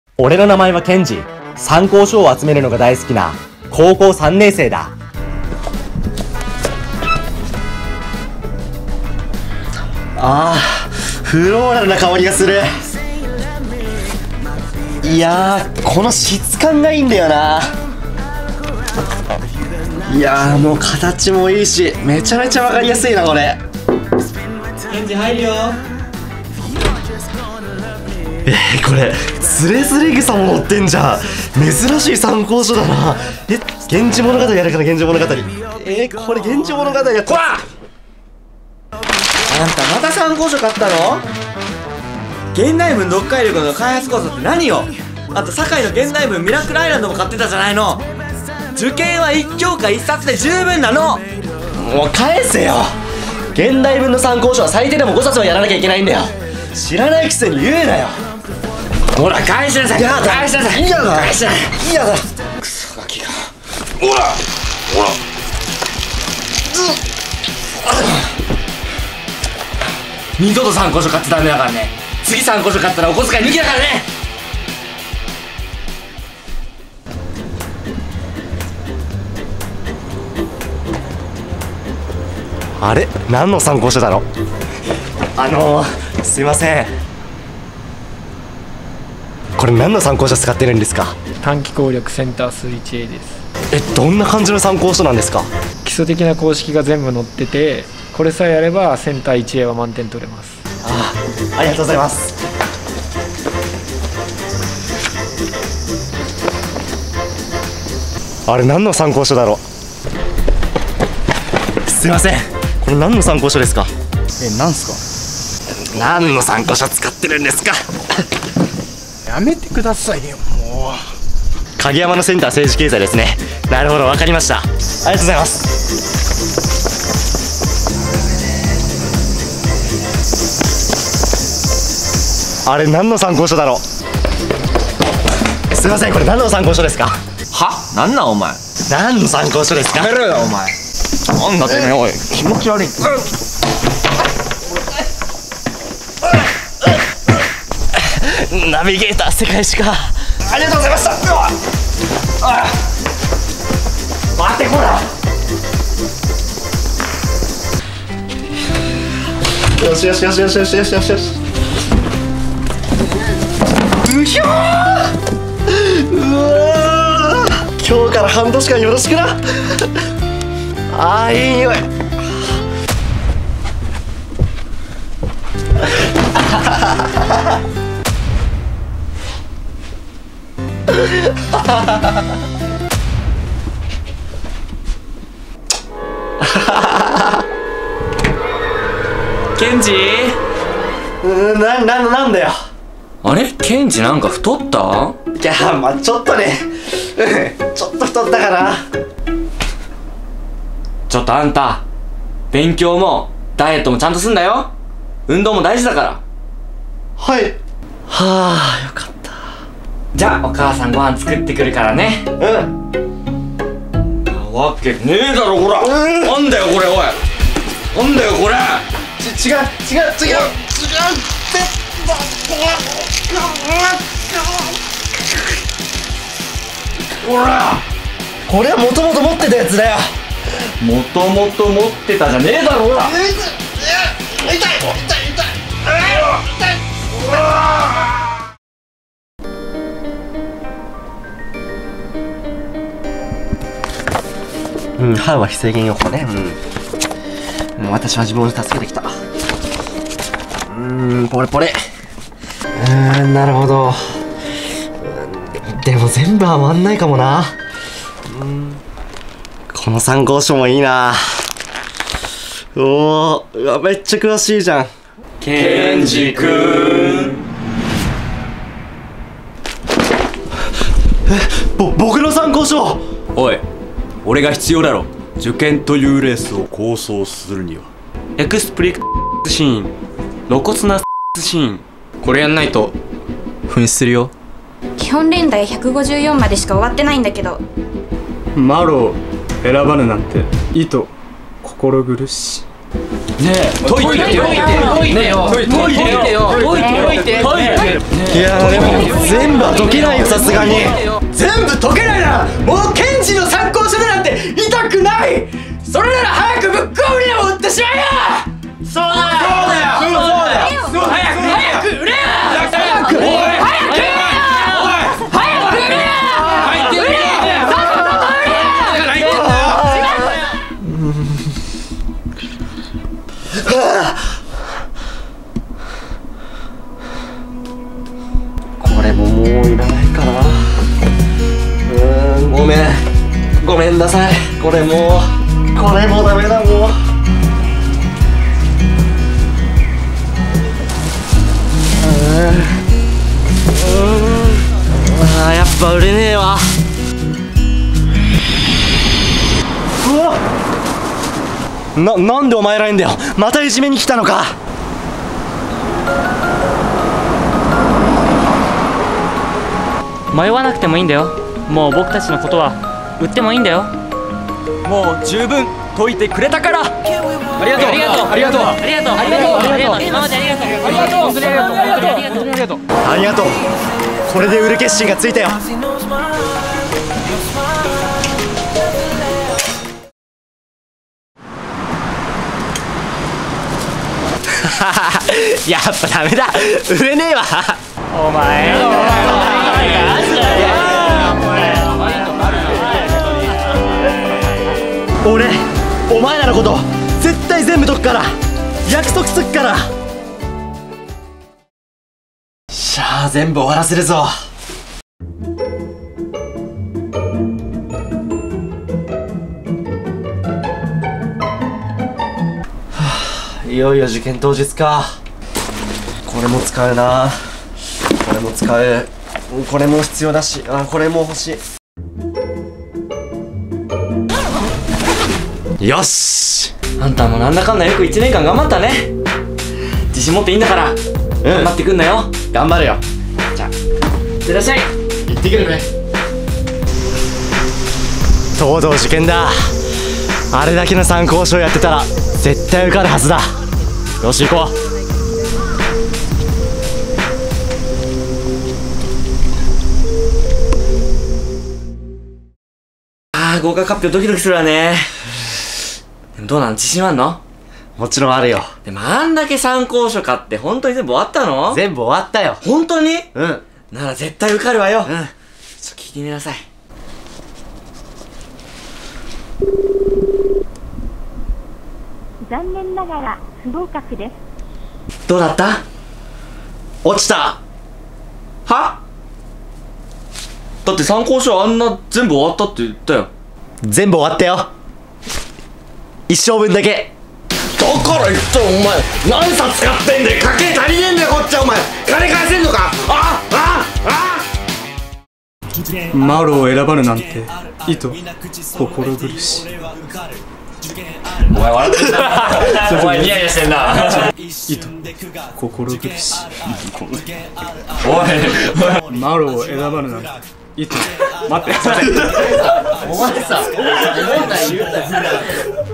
参考書コレクターけんじ
みなさん受験期についつい意味もなく参考書を手に取ってしまうことはありませんでしたか？
そうであったらあなたも「参考書コレクターけんじ」かもしれません。